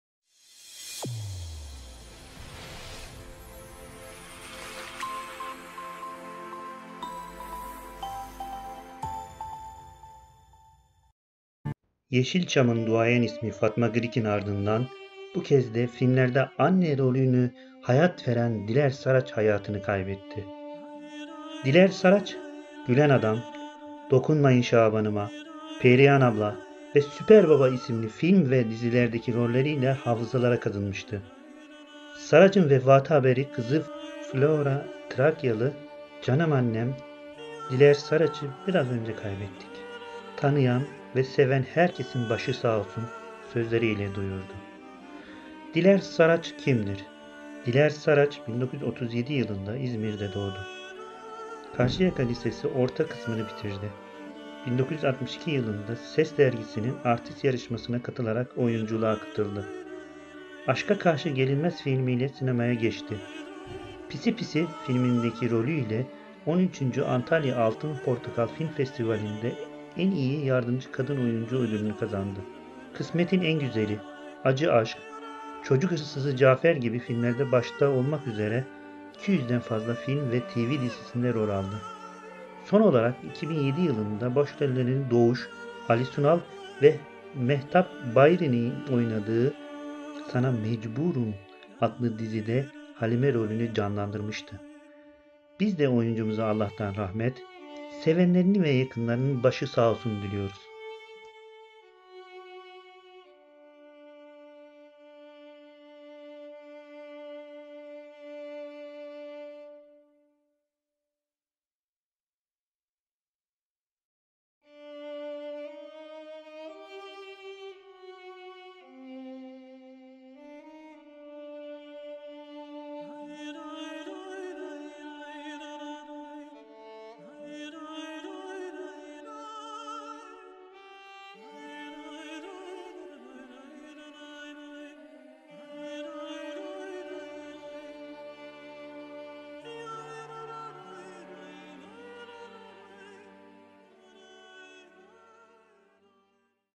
Yeşilçam'dan Bir Acı Haber Daha
Yeşilçam'ın duayen ismi Fatma Girik'in ardından bu kez de filmlerde 'anne' rolüne hayat veren Diler Saraç hayatını kaybetti.

Diler Saraç, "Gülen Adam", "Dokunmayın Şabanıma", "Perihan Abla" ve "Süper Baba" isimli film ve dizilerdeki rolleriyle hafızalara kazınmıştı. Saraç'ın vefat haberini kızı Flora Trakyalı, "Canım annem Diler Saraç'ı biraz önce kaybettik. Tanıyan ve seven herkesin başı sağolsun.." sözleriyle duyurdu.

DİLER SARAÇ KİMDİR?
Diler Saraç, 1937 yılında İzmir'de doğdu. Karşıyaka Lisesi orta kısmını bitirdi. 1962 yılında Ses Dergisi'nin Artist Yarışması'na katılarak oyunculuğa katıldı. Aşka Karşı Gelinmez filmiyle sinemaya geçti. Pisi Pisi (1975) filmindeki rolüyle 13. Antalya Altın Portakal Film Festivali'nde En İyi Yardımcı Kadın Oyuncu Ödülü'nü kazandı. Kısmetin En Güzeli, Acı Aşk, Çocuk Hırsızı Cafer gibi filmler başta olmak üzere 200'den fazla film ve TV dizisinde rol aldı. Son olarak 2007 yılında başrollerde Doğuş, Ali Sunal ve Mehtap Bayri'nin oynadığı, Sana Mecburum adlı dizide Halime rolünü canlandırmıştı.
Bizde oyuncumuza Allah'tan rahmet sevenlerinin ve yakınlarının başı sağolsun diyoruz.